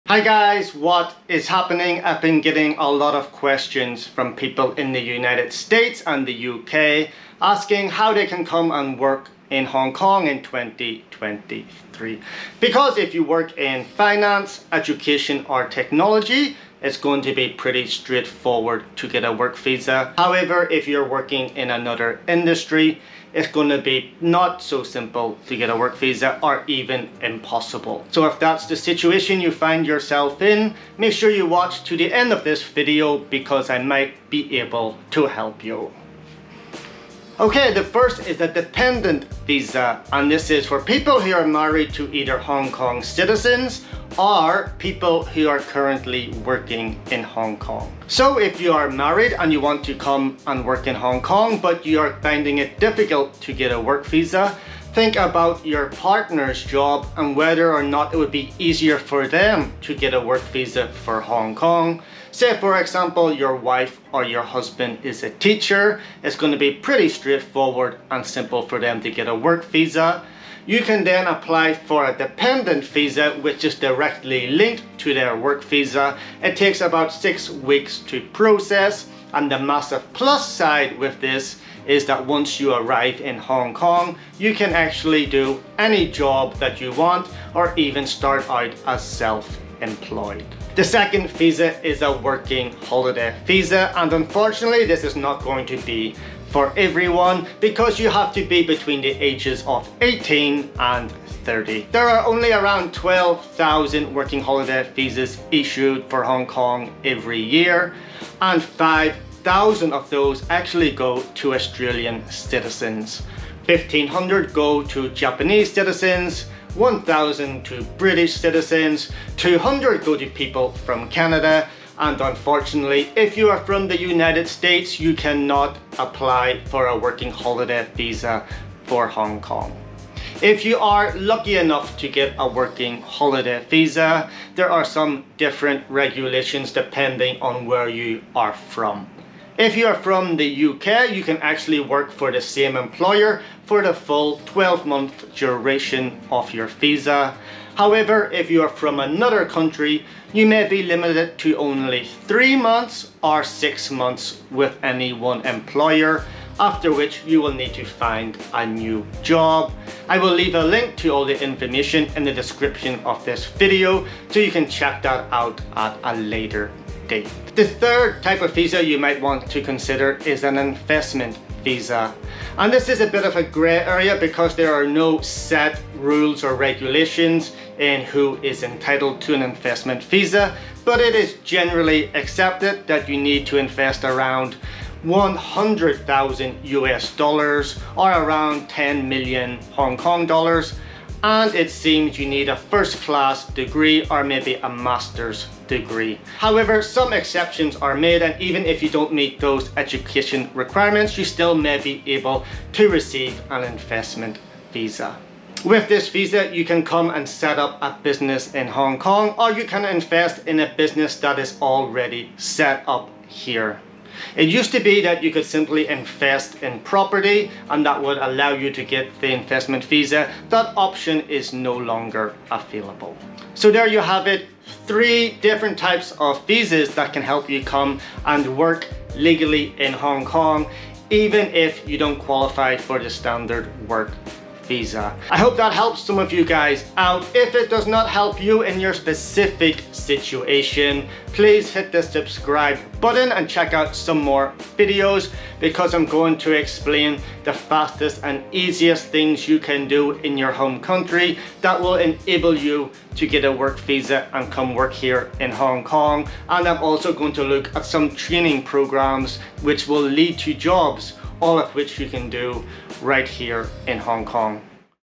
How to work (legally) in Hong Kong 2023 (without a work visa)
How to get a job and work legally in Hong Kong in 2023, even if you don't qualify for a work visa. Information on how you can live and work in Hong Kong.